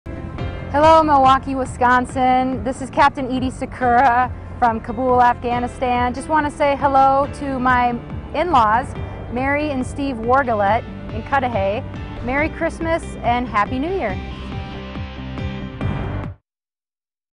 Military Greetings: Eydie from Cudahy
Captain Edyie Sakura is in Kabul, Afghanistan.